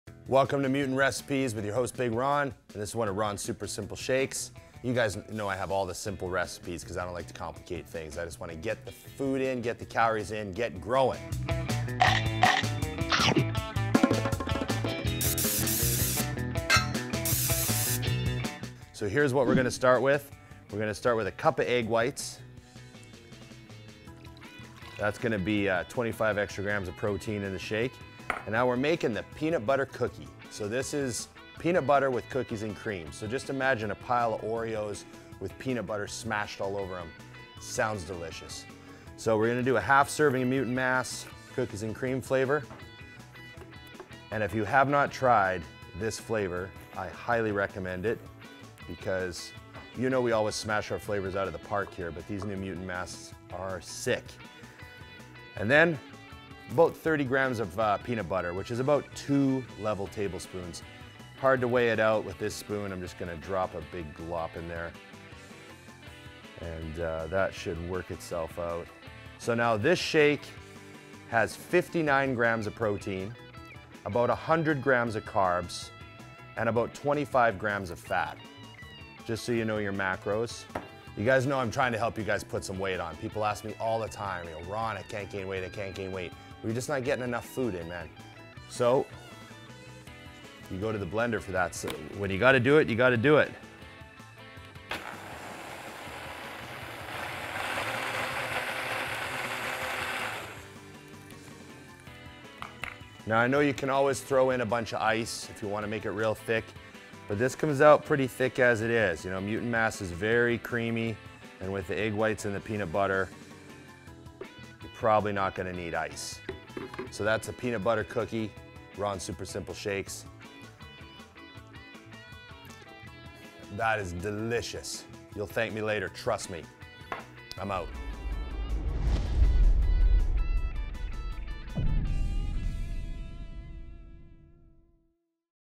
MUTANT RECIPES - MUTANT MASS Peanut Butter Cookie Shake
MUTANT RECIPES - MUTANT MASS Peanut Butter Cookie Shake. This is definitely a big shake, full of amazing flavor and hard hitting macros. IFBB Pro Ron Partlow knows how to load up the calories.

Protein - 59g
Carbs - 100g
Fats - 25g

So get this shake going if you can't gain weight. It might be what you need to move the scale. MUTANT RECIPES - MUTANT MASS Peanut Butter Cookie Shake